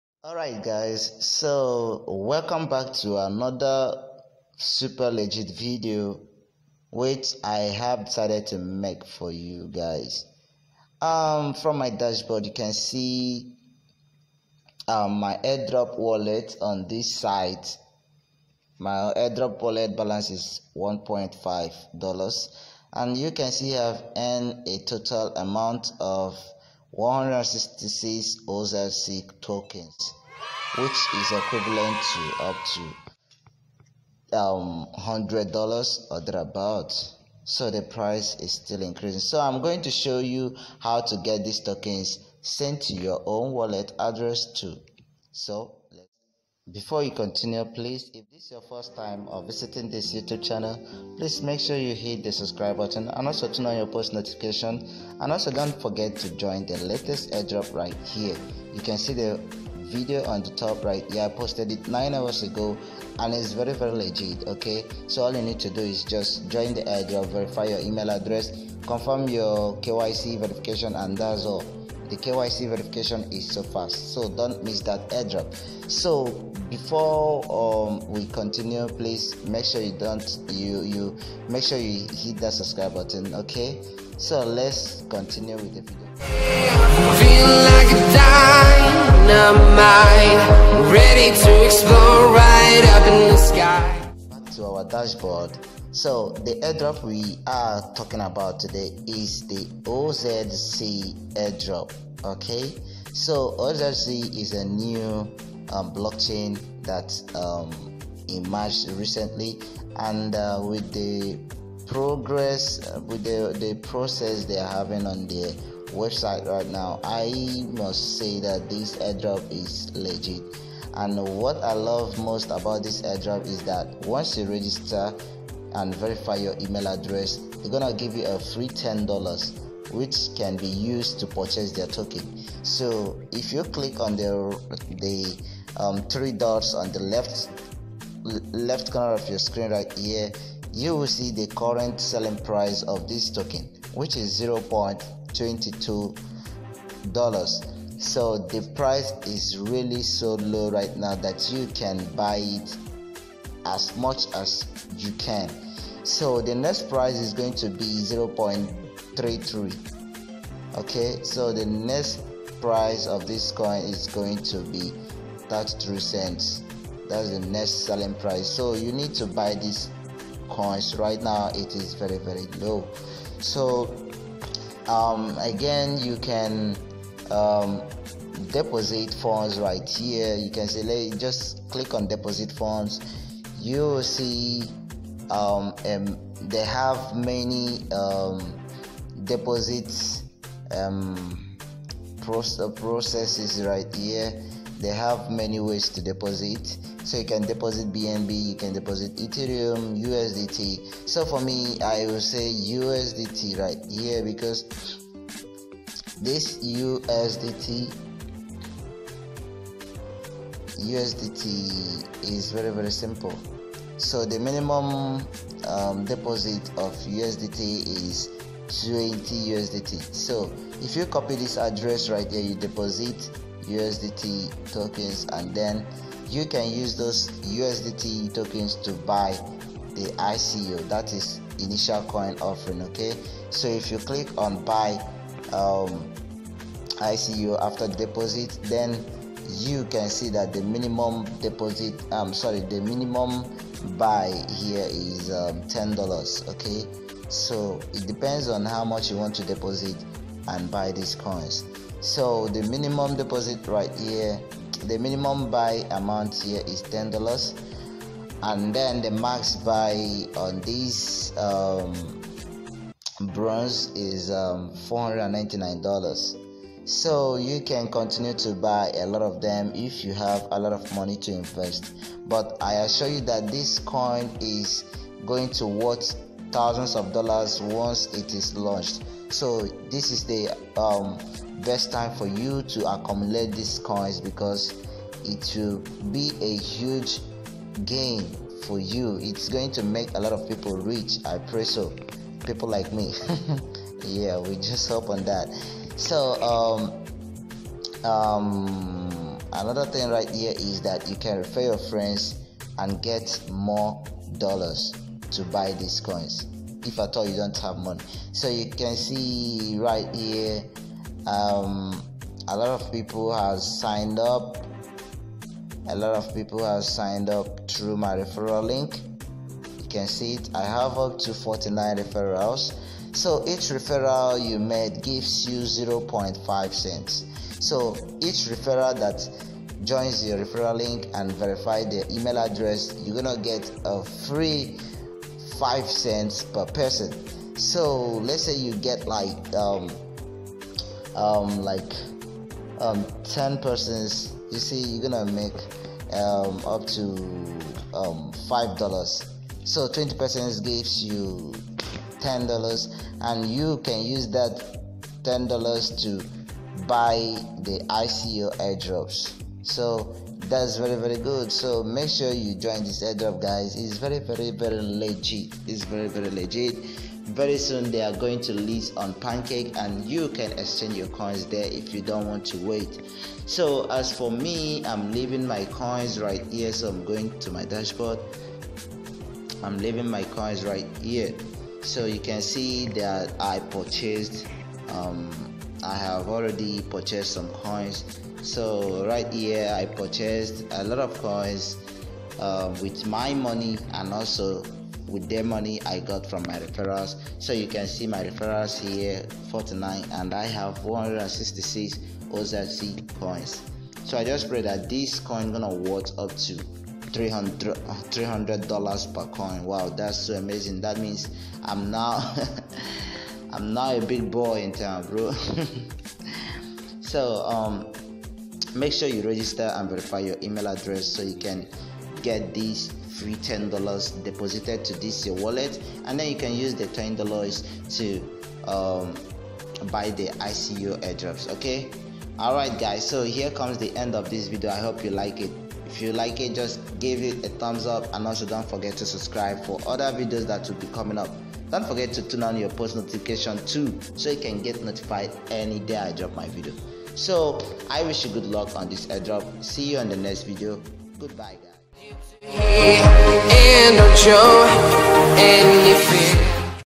OZC Token Airdrop | Claim Free $10 To Your OZC Wallet | New Airdrop
OZC airdrop is a new coin and has its brockchain to be listed on exchanges soonest; , register and join the ICO program